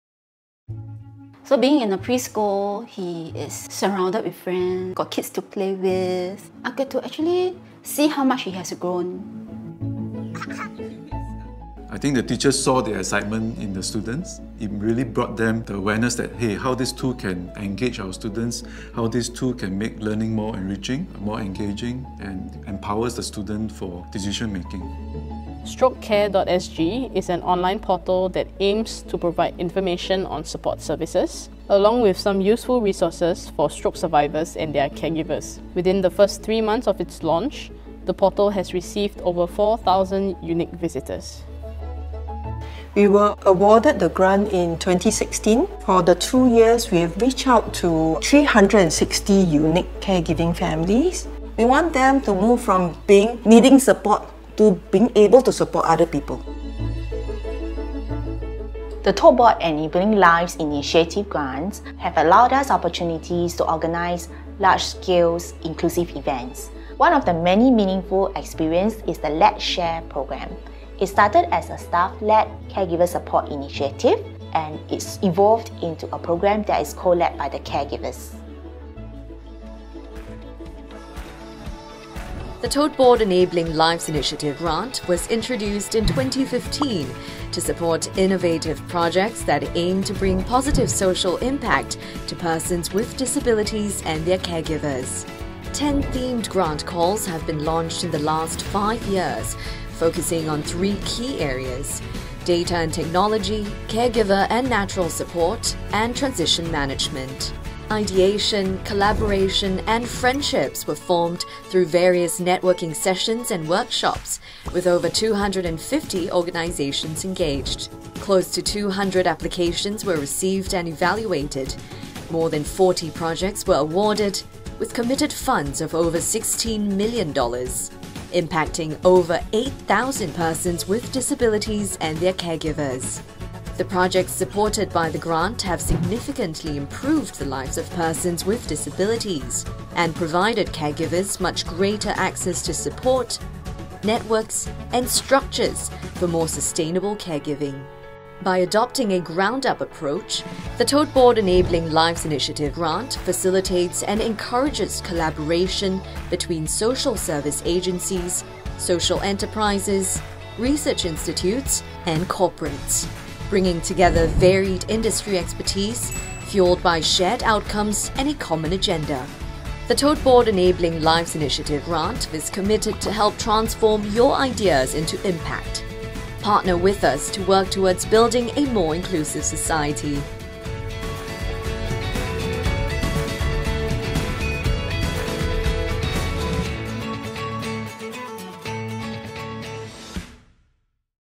Tote Board Enabling Lives Initiative (TBELI) Grant [2015 – 2020]
Funded by Tote Board and administered by SG Enable, over $16 million was committed under Tote Board Enabling Lives Initiative (TBELI) Grant [2015 – 2020] through 10 themed grant calls, supporting over 40 projects and initiatives, in three cross-cutting disability areas of Data and Technology, Caregiver and Natural Support, and Transition Management.